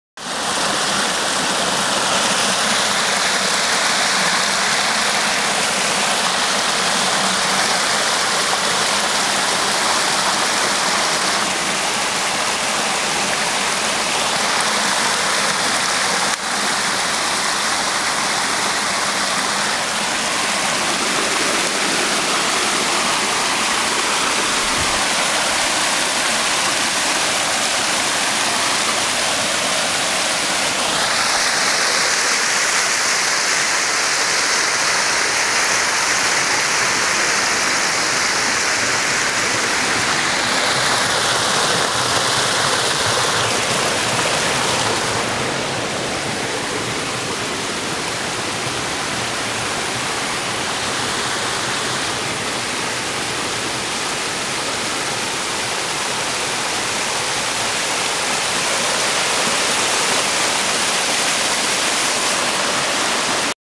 Bardstown manhole overflowing in the Community Park 6-22-09
We had  a thunderstorm this morning and as you can see the capacity of the sewer line was compromised by water inflow/infiltration resulting in a wet weather SSO.  This is on the Town Creek Line